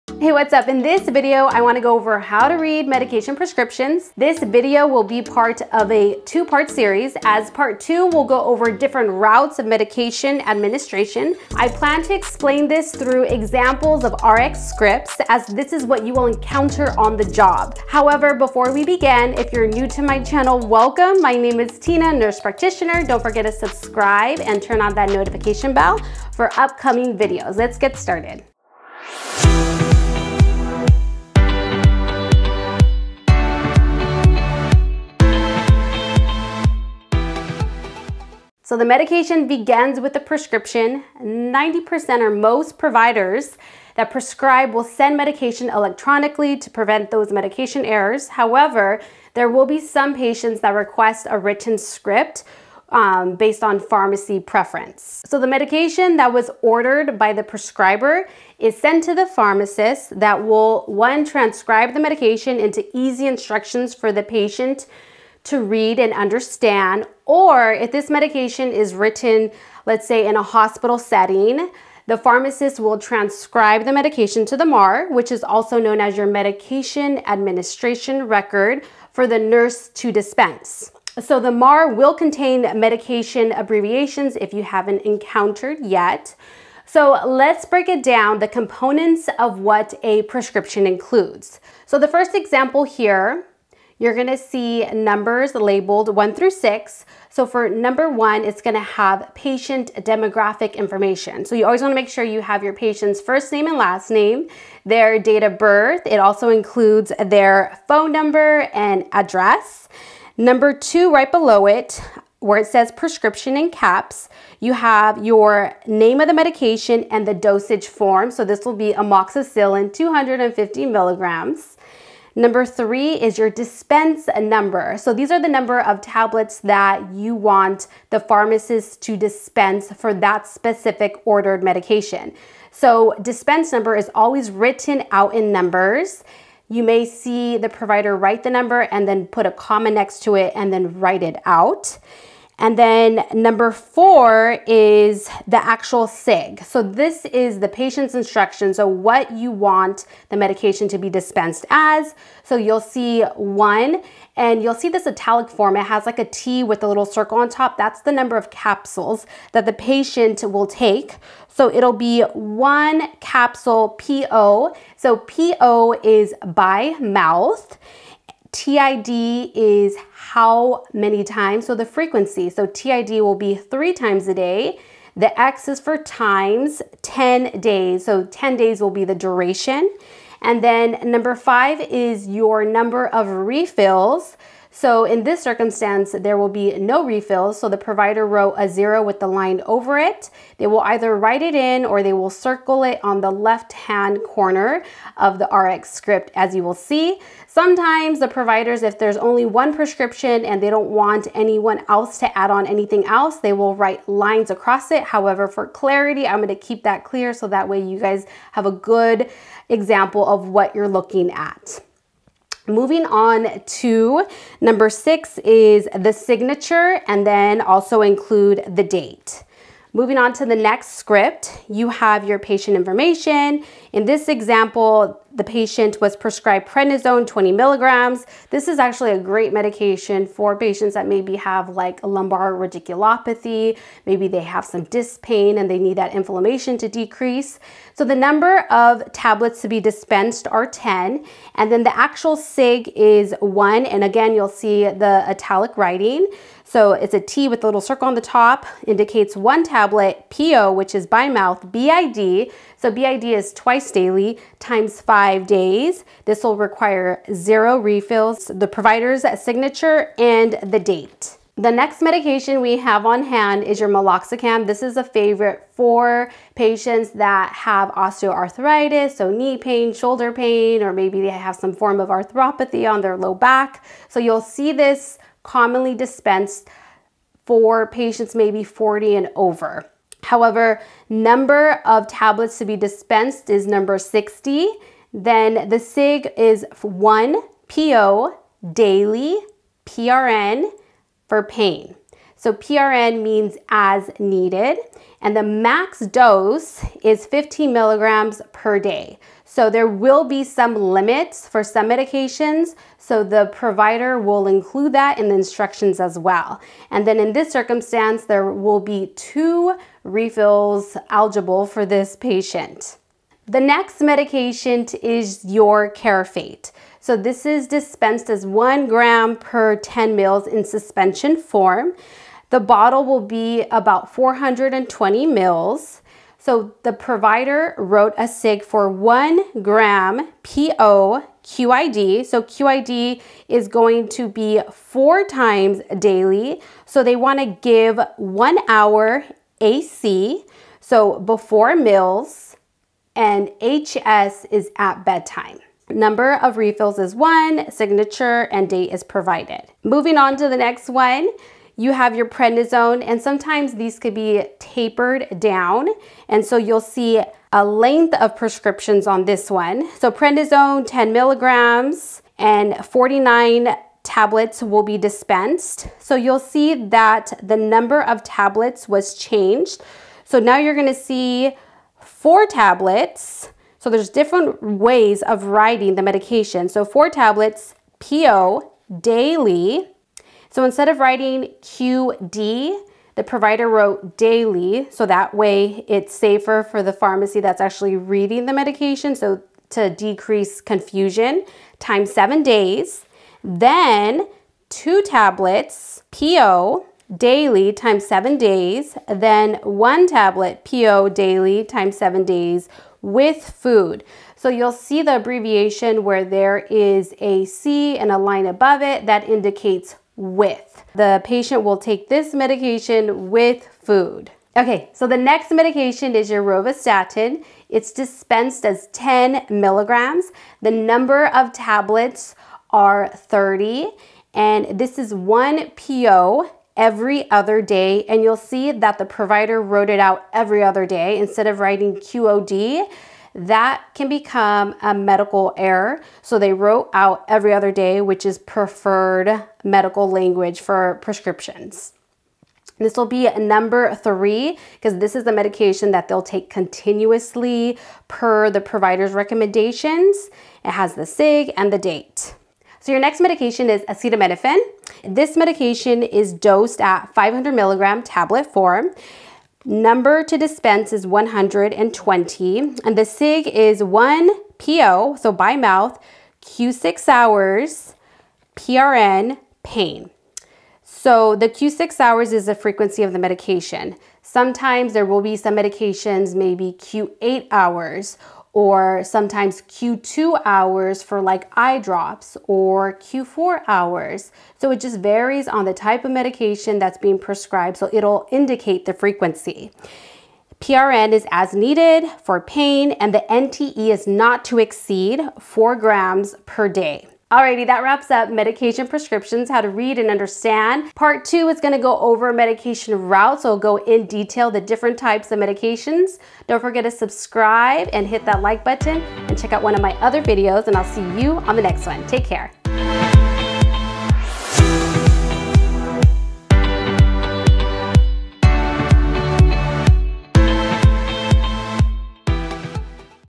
How To Read a Doctor Prescription Part 1 with medication abbreviations
In this video I am going to go over parts of a medication prescription with a review on medical abbreviations. This is a great introduction for pharmacology review which is a 2 part series. Part 2 will go over the route of medication administration.

Don't forget to checkout my links below, and ill catch you on the next on

Here is the link to find out more about me and what I offer: